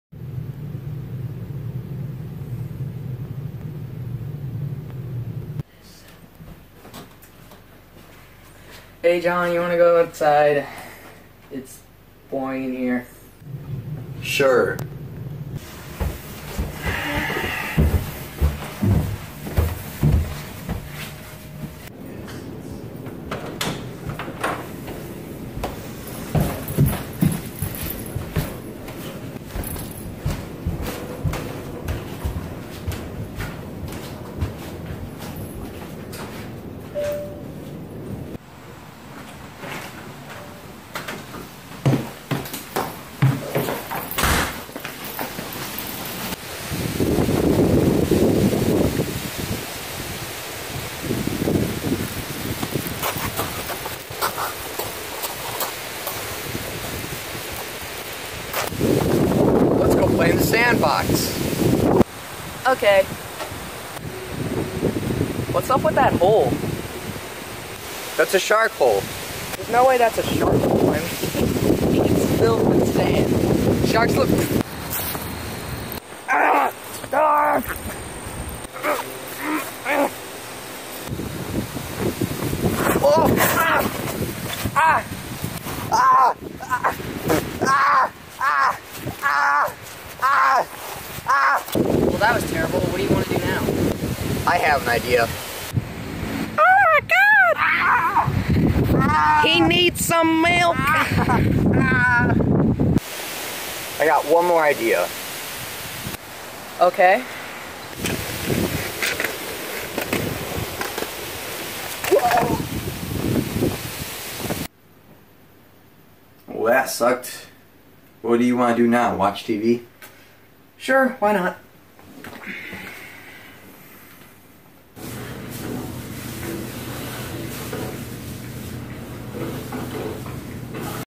Shark hole
Me and my friend Conor are bored and try to find activities to do.